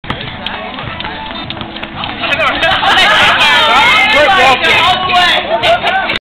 AVP Year End Event: The Cookout Spring 2009 11
Angelic Voice of Praise celebrate the year w/food, fellowship and fun